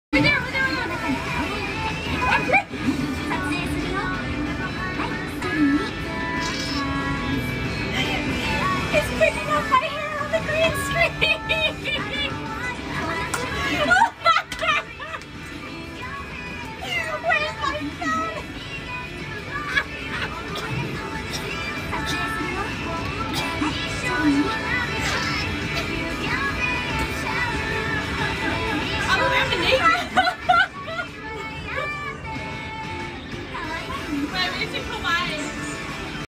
Meowri & Batz- Laughing, Choking, Posing
Just making some funny faces in the photo booth. And completely losing it.

for more silliness and sexiness, follow Jenna